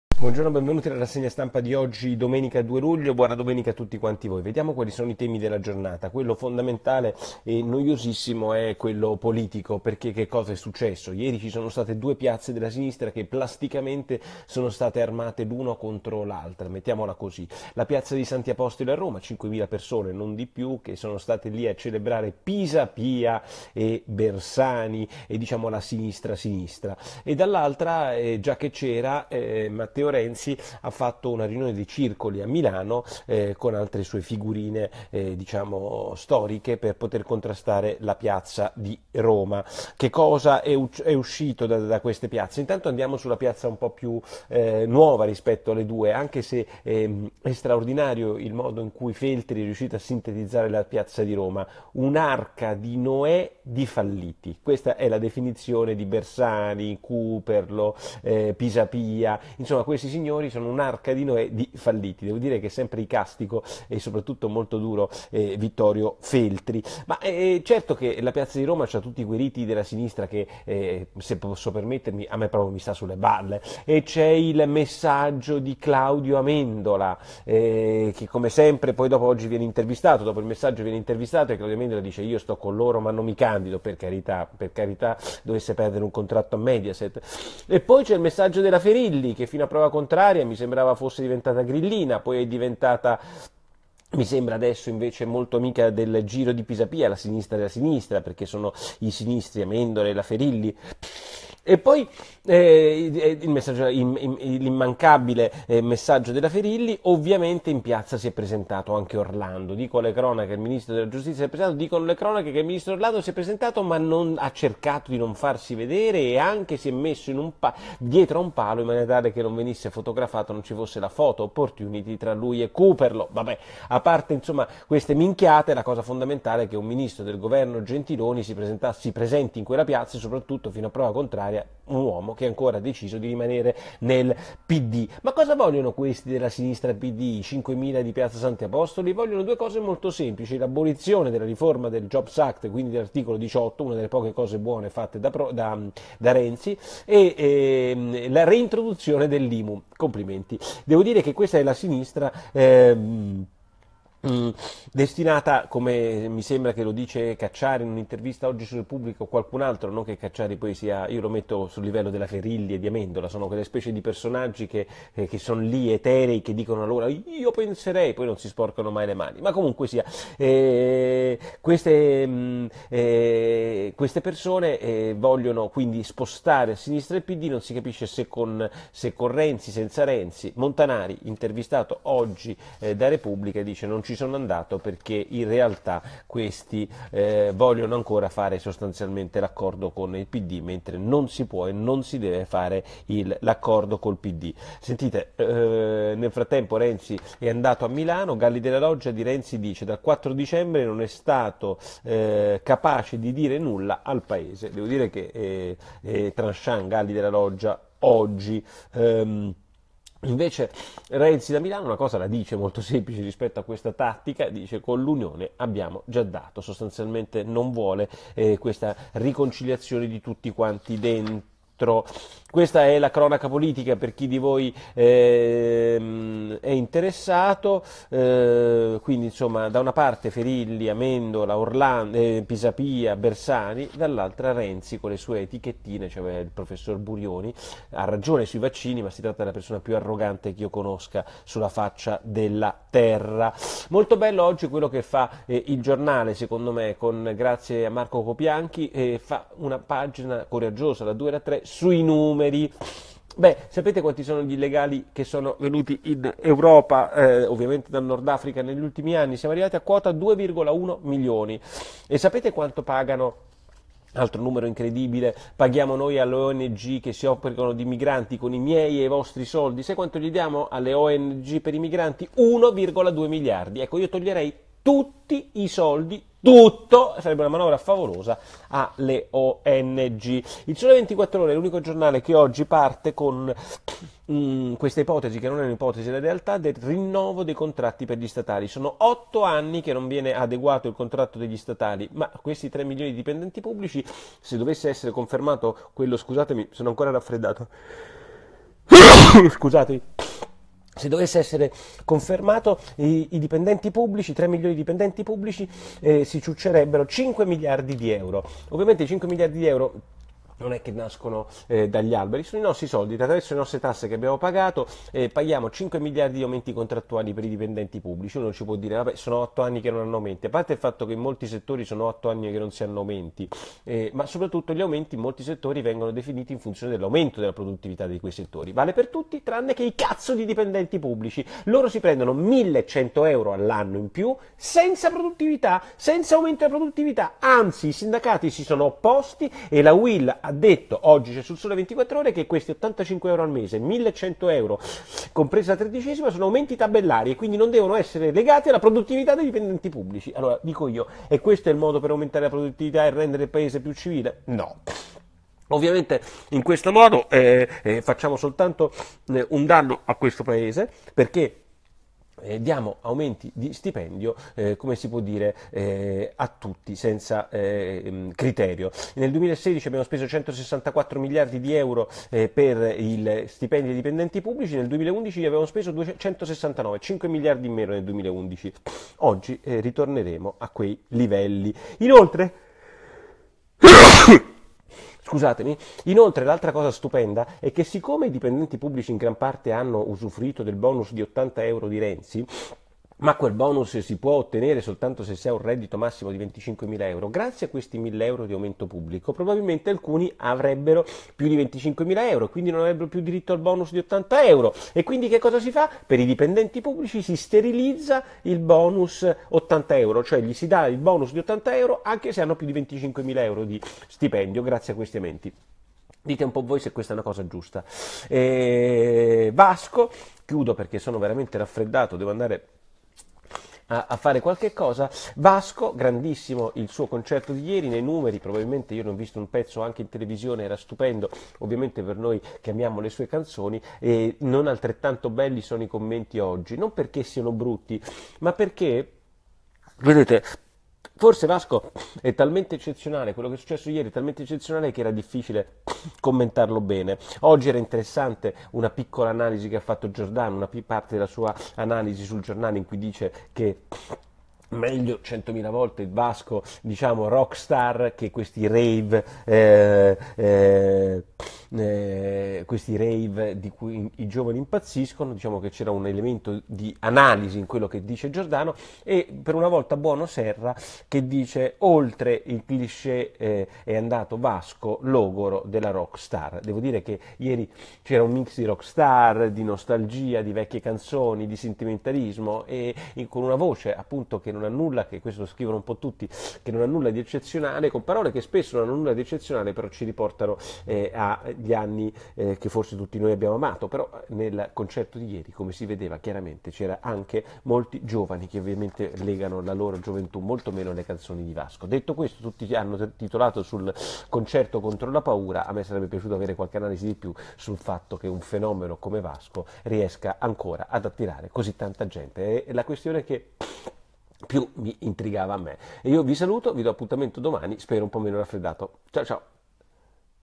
Clandestini, Ong e rinnovo statali. Chi paga? Noi, of course (2 luglio 2017)
00:00 La sinistra di Pisapia Bersani, Amendola e Ferilli.
03:15 La sinistra di Renzi (la persona più arrogante che ho conosciuto)
04:05 Immigrazione, chi paga? Noi
04:40 Rinnovo degli statali, chi paga? Sempre noi
07:00 Vasco super, commenti cosí cosí